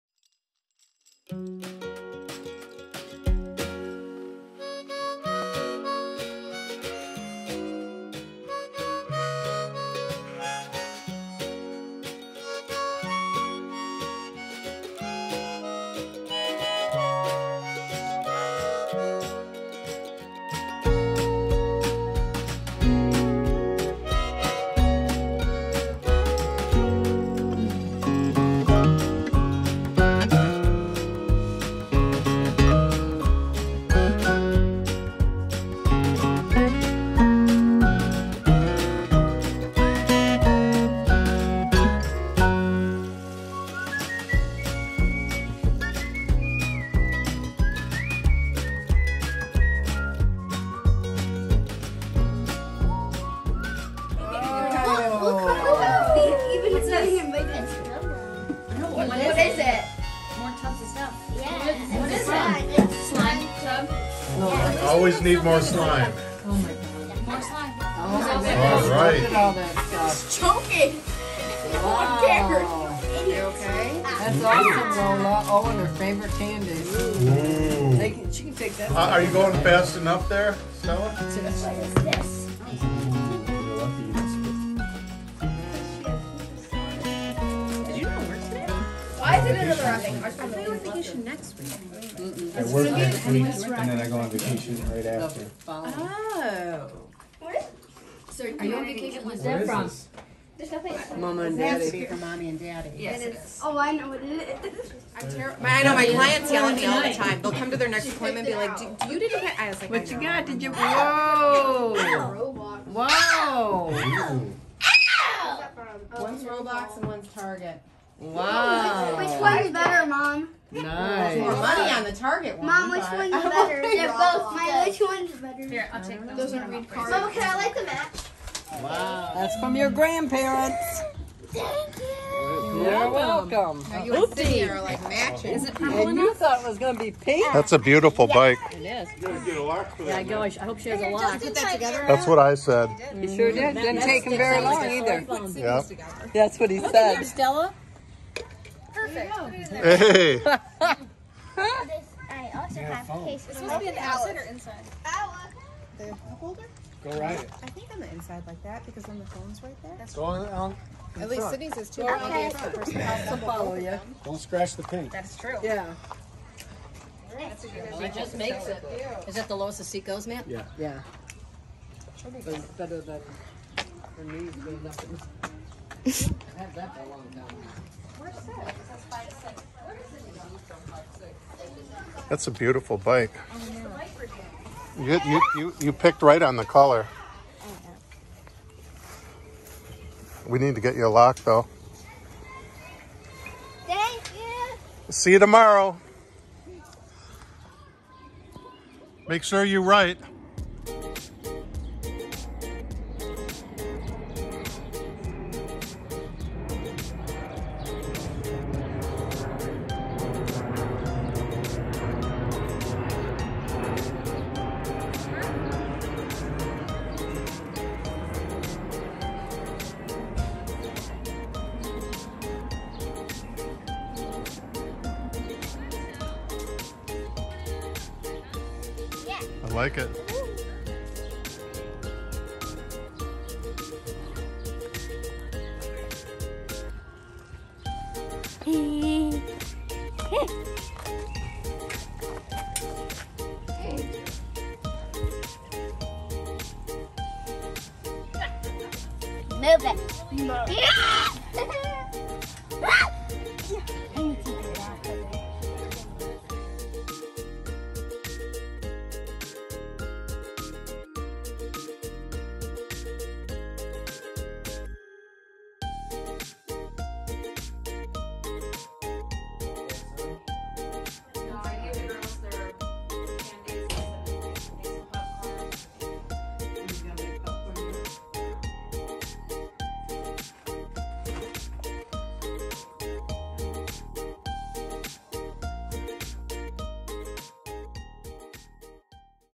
Stella Turn’s 10
Stella finally reached double digits, 10 years old.